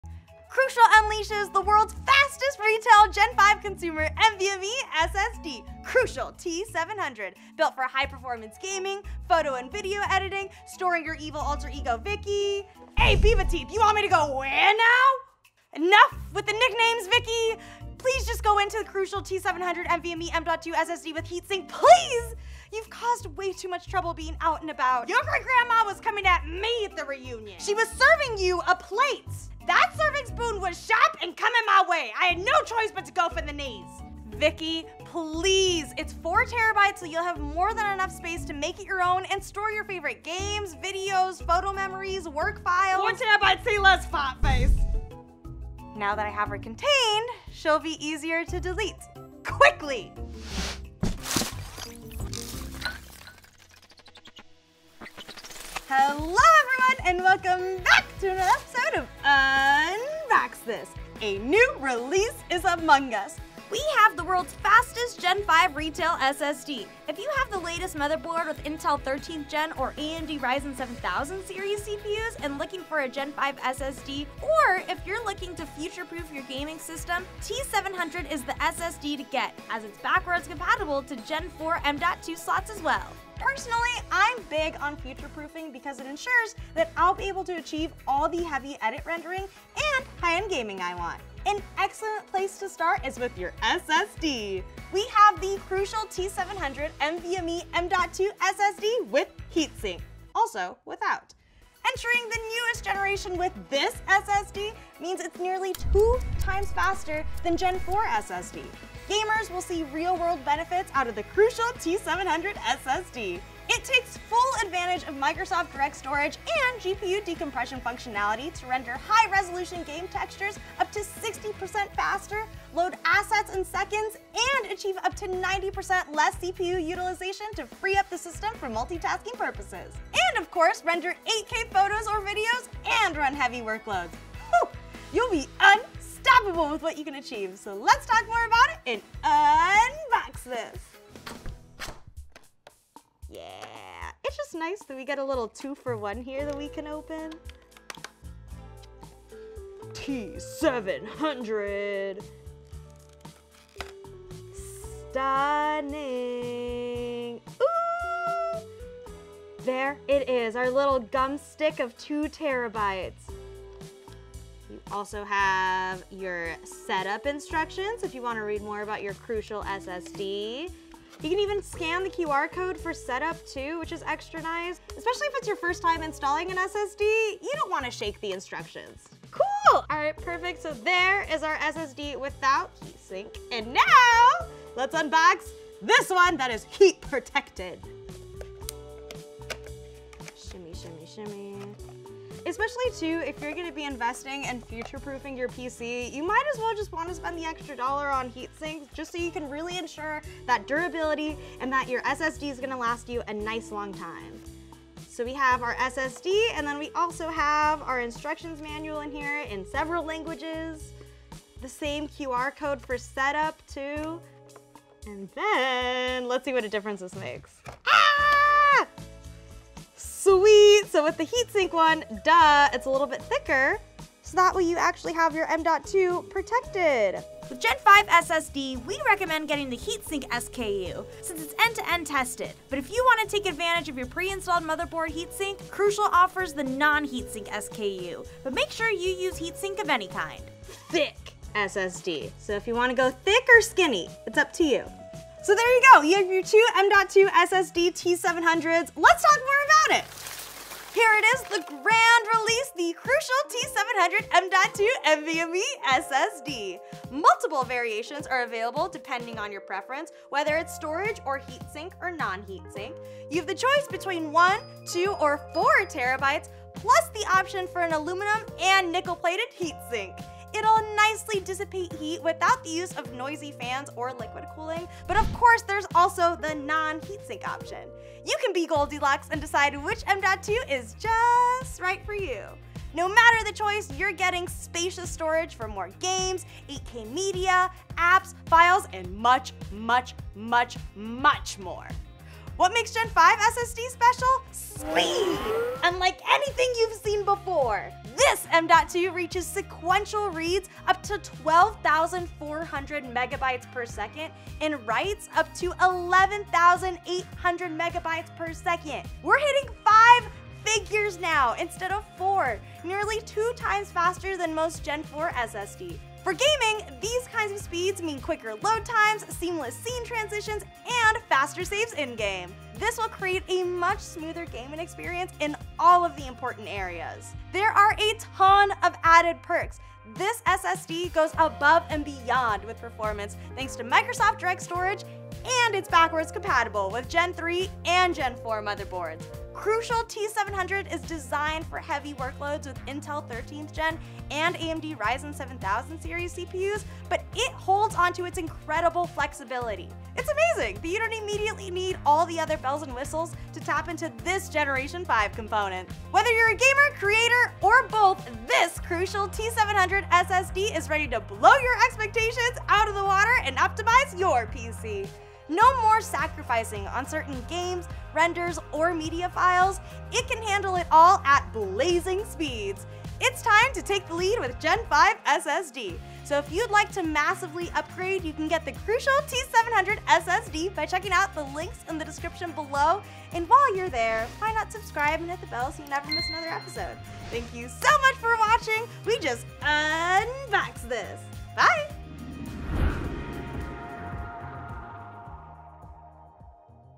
UPGRADE TO THE LATEST HIGH SPEED SSD Crucial T700 - Unbox This!
The latest M.2 SSD is HERE! Crucial T700 is storming in with a powerful and speedy line of SSD! 1TB, 2TB, and 4TB with or without heatsink! The choice is YOURS! Regardless, you'll be able to achieve blistering read/write speeds, faster renders, reduced load times, and wide storage! Don't let last gen storage slow you down, upgrade today!